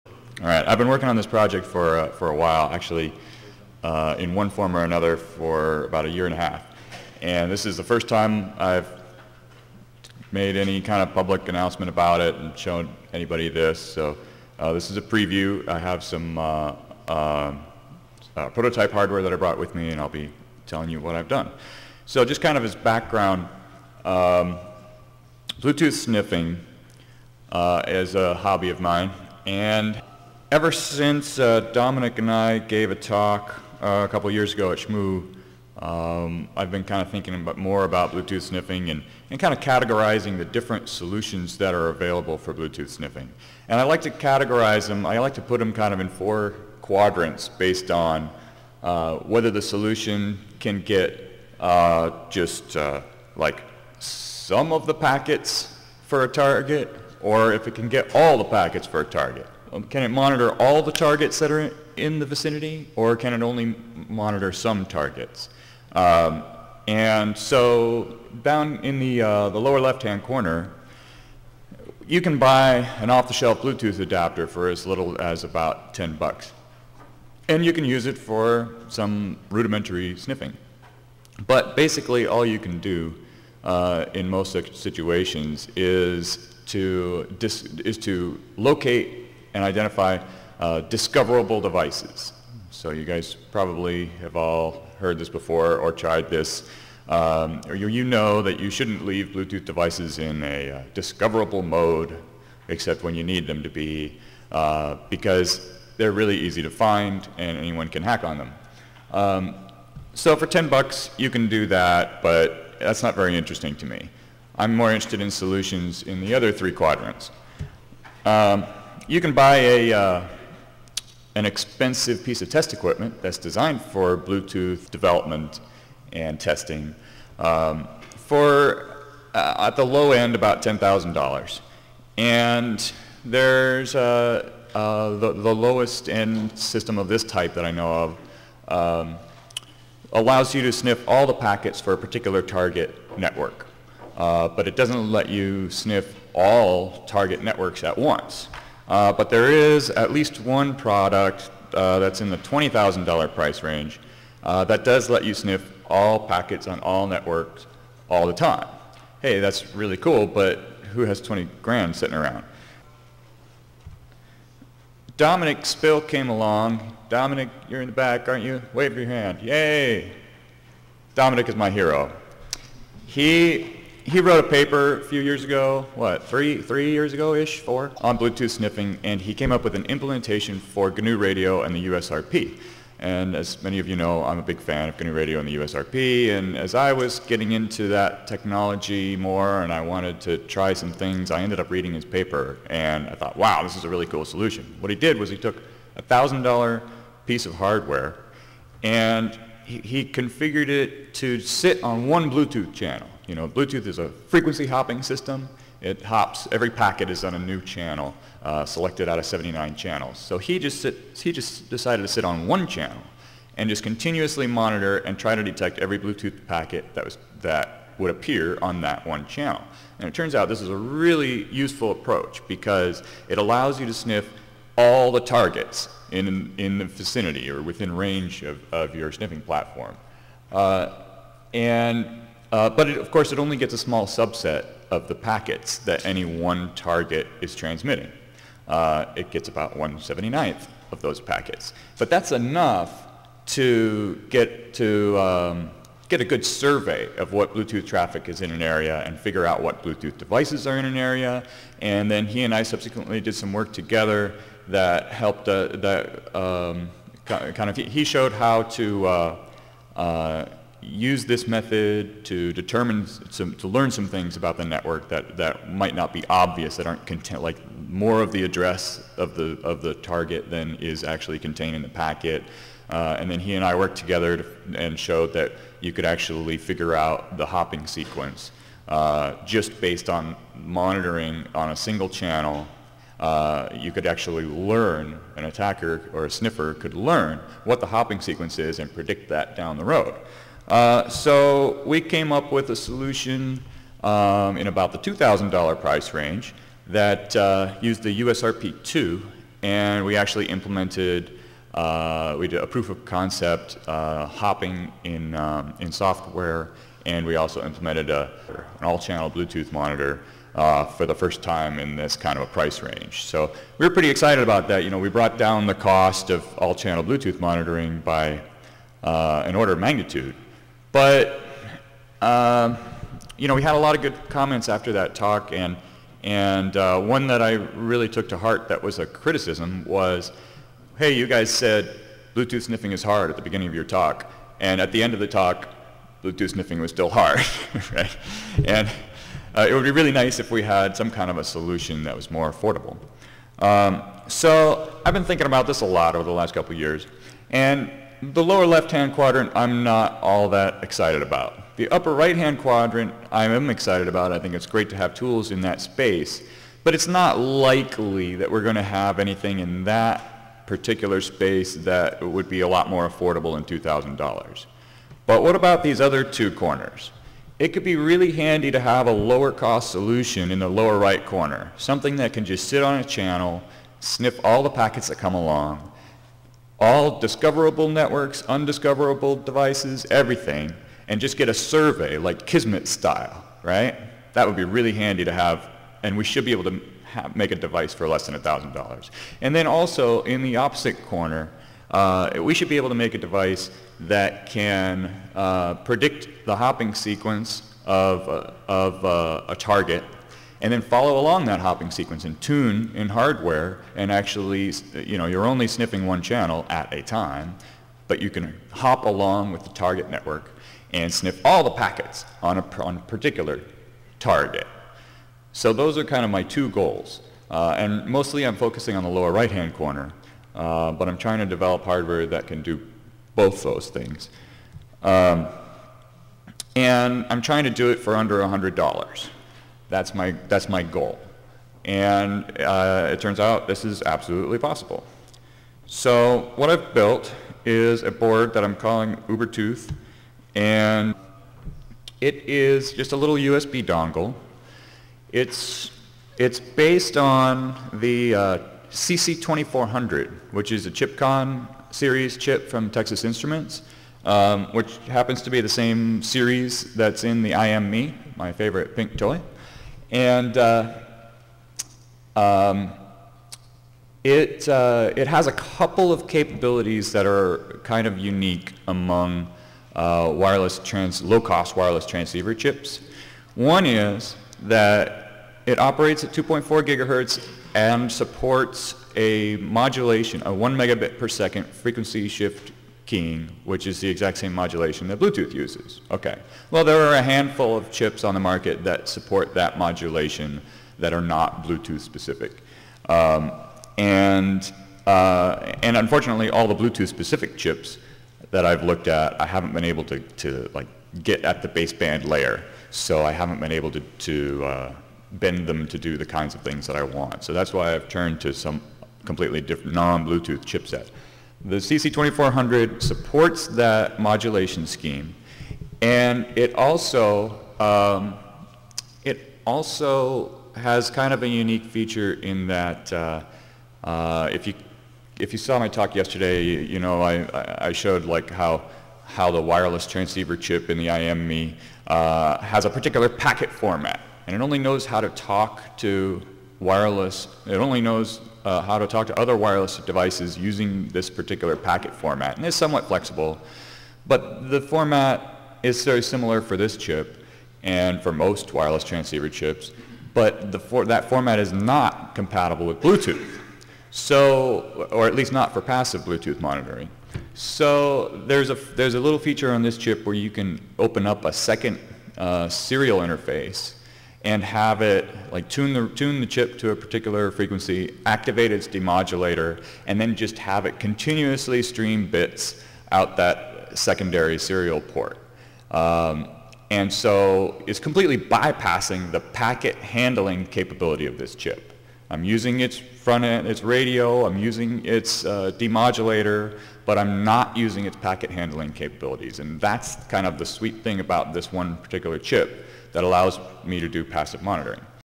ToorCon 12: Ubertooth Zero, a preview (part 1 of 2)
Michael Ossmann presents a preview of a new open source hardware platform that can be used for Bluetooth sniffing and development at ToorCon 12 (2010).